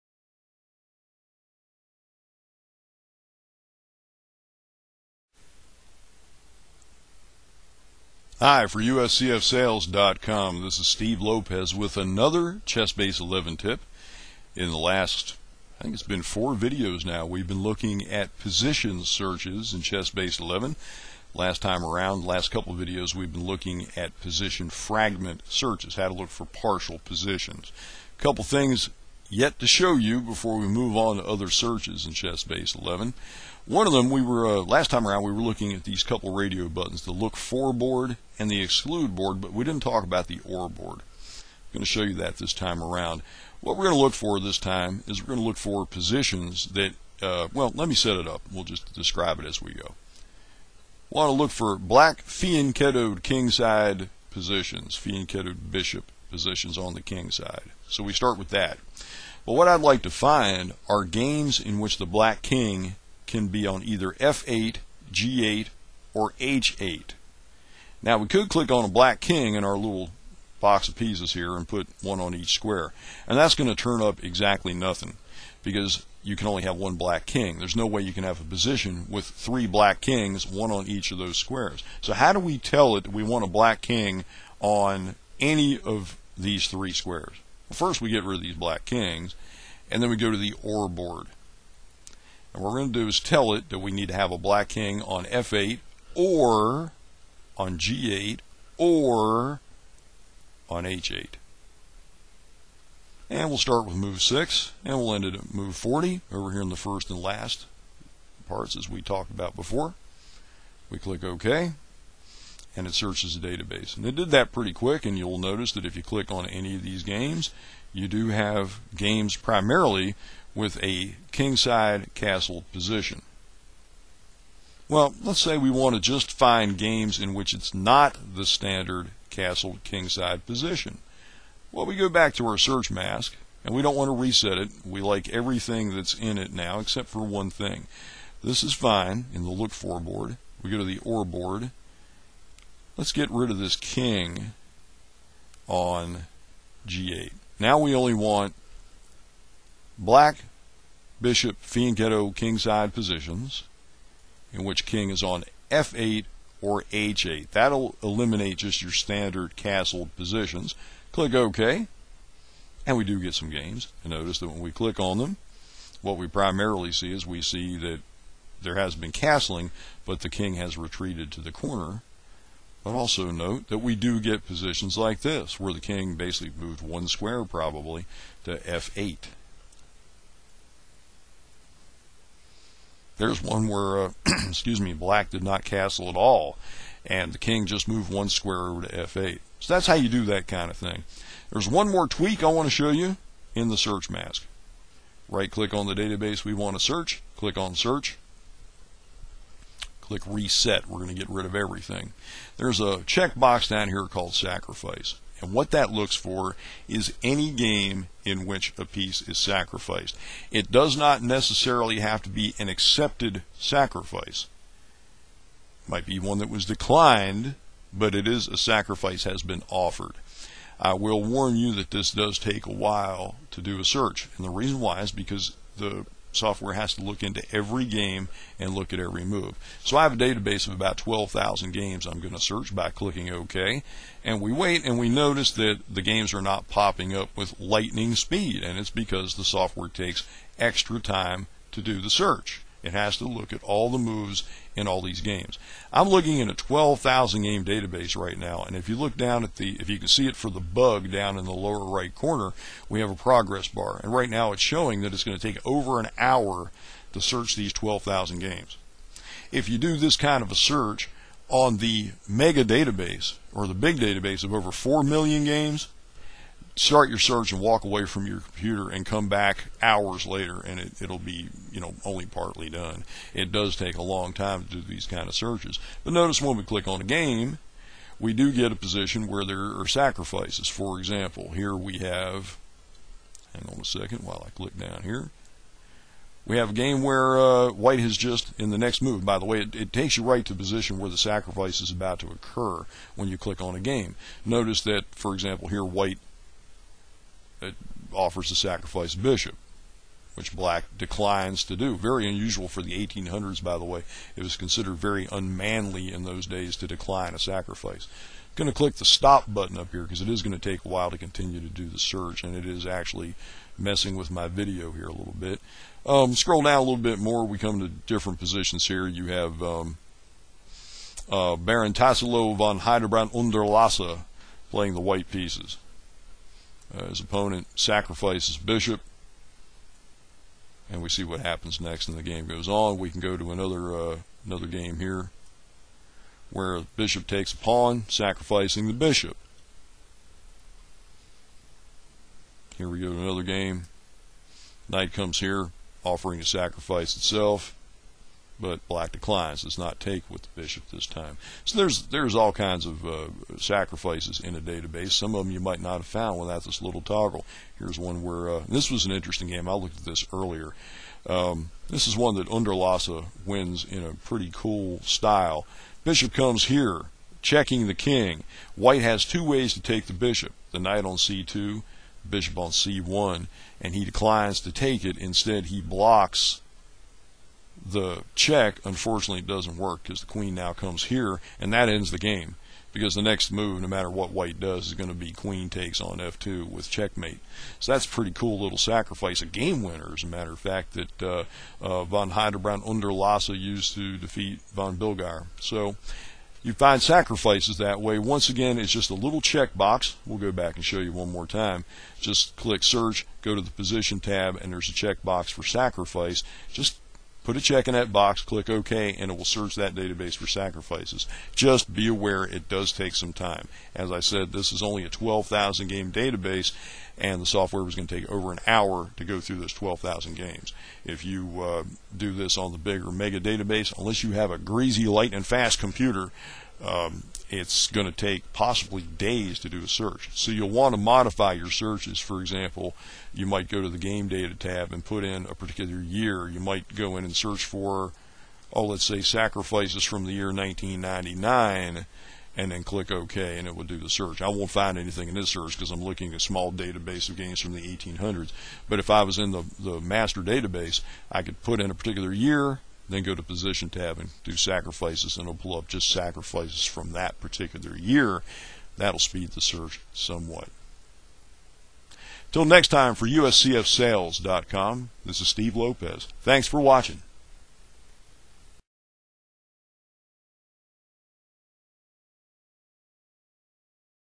Finding chess games with piece sacrifices in a database (ChessBase Tip #0009)
USCF Sales ) brings you another ChessBase chess software tip, presented by Steve Lopez (a renowned chess software authority for two decades, also currently blogging Steve illustrates some "hidden" features of chess position searches in ChessBase 11, including "Or" board searches and piece sacrifice searches.